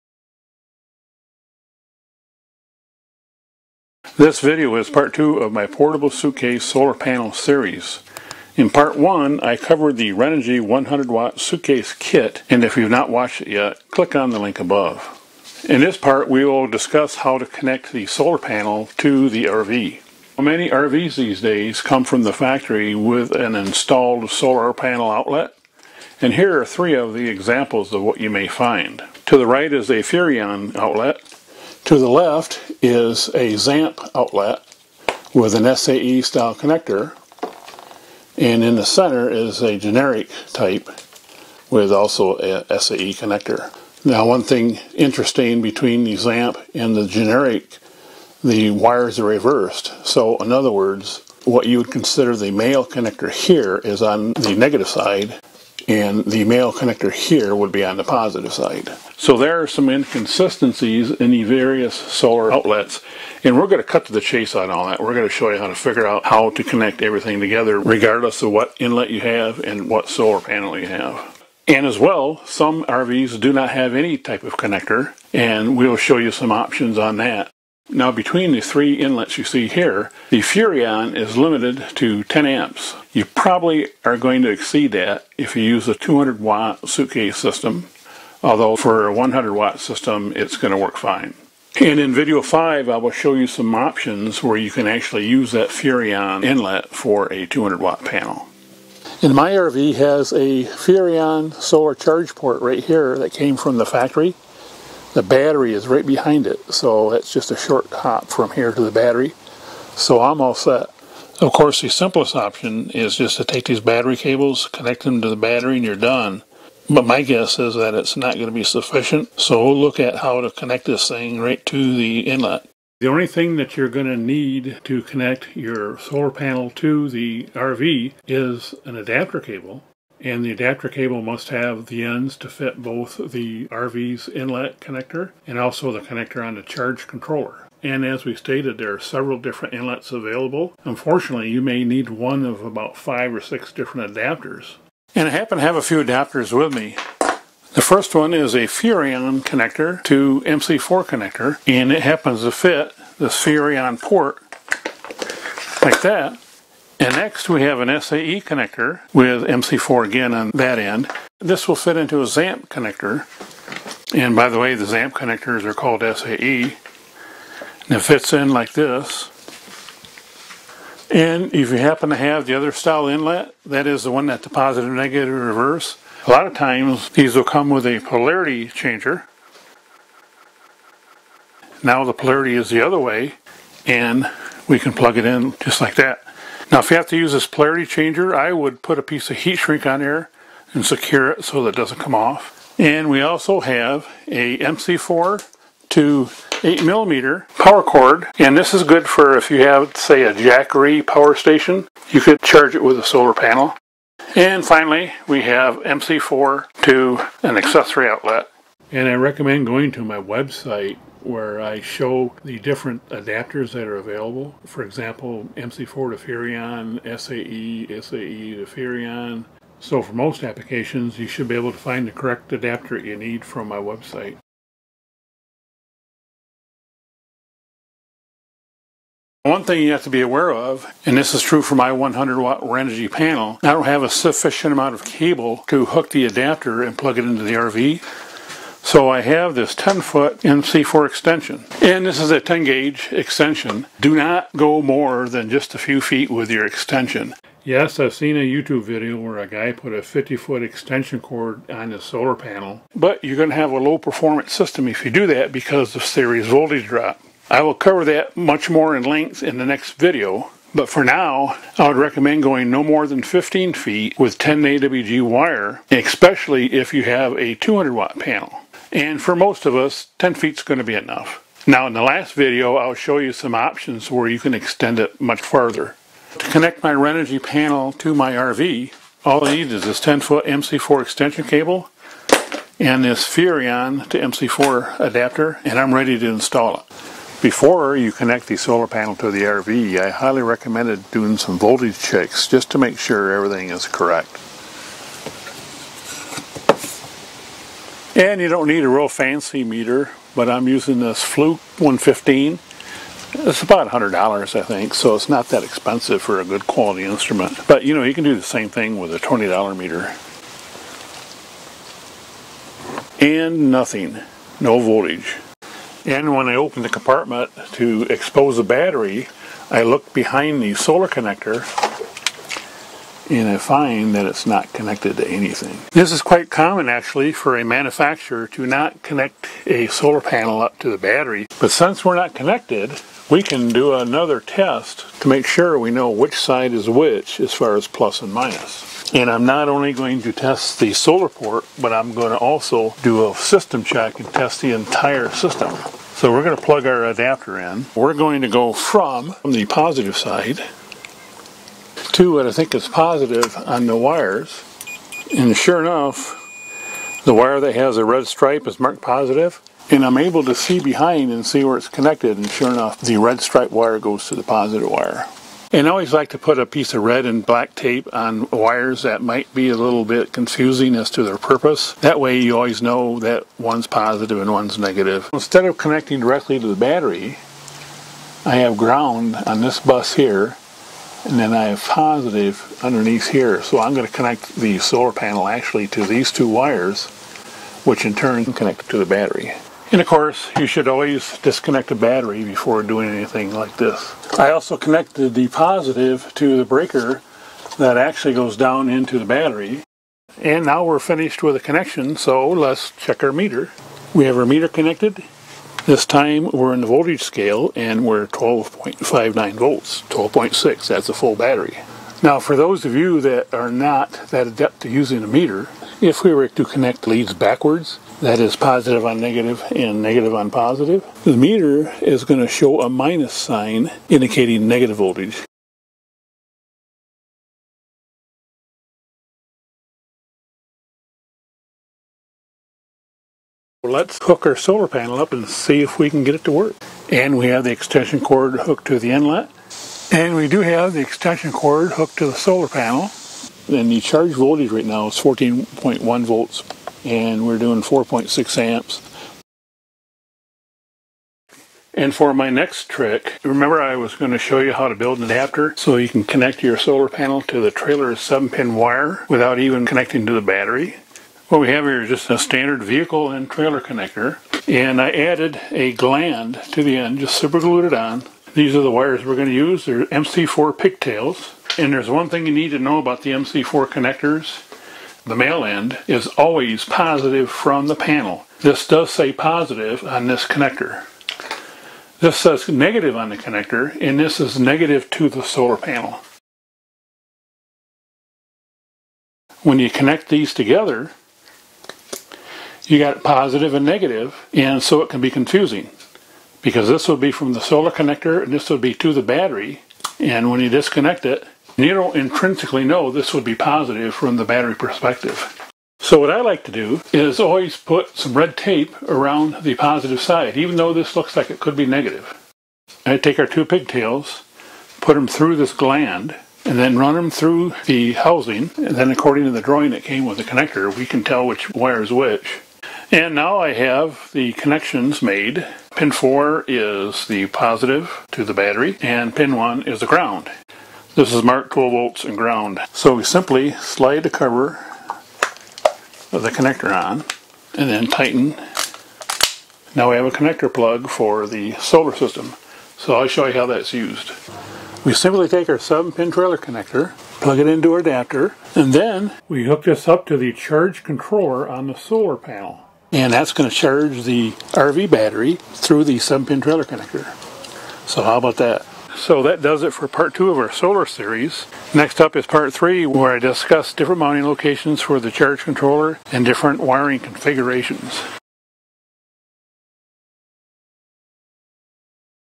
Connecting a Suitcase Solar Kit to a RV - Portable RV Solar Charging Video 2
In this video, we explore options in connecting the Suitcase Solar Panel Kit to  my Grand Design Reflection 29RS fifth wheel RV.

In this 2nd video of my RV Portable Suitcase Solar Charging series, we show how to connect a portable Solar Panel to your RV - especially given many RVs come from the factory these days with either a Furrion or Zamp/SAE style solar connector.

Unfortunately, this typically requires an adapter as each of the solar connector offerings in the RV world are engineered to get you to buy their panels - which can be more expensive than others.